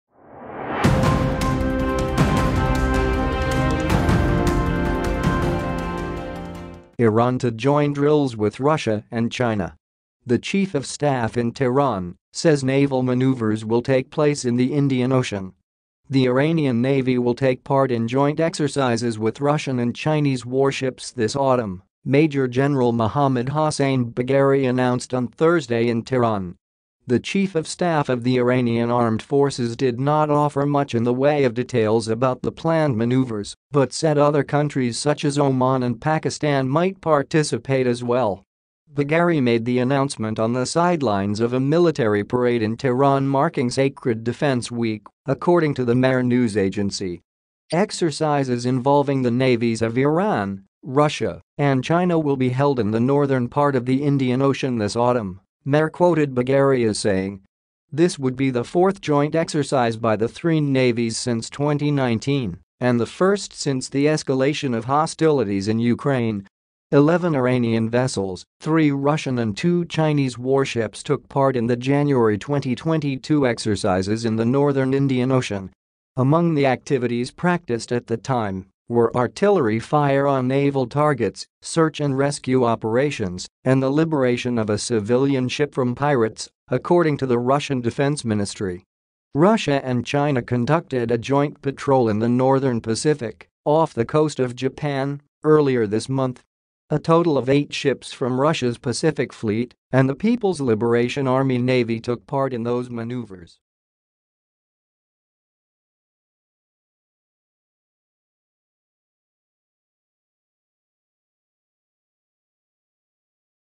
Iran to join drills with Russia and China.
The chief of staff in Tehran says naval maneuvers will take place in the Indian Ocean.

The Iranian navy will take part in joint exercises with Russian and Chinese warships “this autumn,” Major General Mohammad Hossein Bagheri announced on Thursday in Tehran.

The chief of staff of the Iranian armed forces did not offer much in the way of details about the planned maneuvers, but said other countries such as Oman and Pakistan might participate as well.

Bagheri made the announcement on the sidelines of a military parade in Tehran marking Sacred Defense Week, according to the Mehr news agency.

“Exercises involving the navies of Iran, Russia and China will be held in the northern part of the Indian Ocean this autumn,” Mehr quoted Bagheri as saying.

This would be the fourth joint exercise by the three navies since 2019, and the first since the escalation of hostilities in Ukraine. Eleven Iranian vessels, three Russian and two Chinese warships took part in the January 2022 exercises in the northern Indian Ocean. Among the activities practiced at the time were artillery fire on naval targets, search-and-rescue operations, and the liberation of a civilian ship from pirates, according to the Russian Defense Ministry.

Russia and China conducted a joint patrol in the northern Pacific, off the coast of Japan, earlier this month. A total of eight ships from Russia’s Pacific Fleet and the People’s Liberation Army Navy took part in those maneuvers.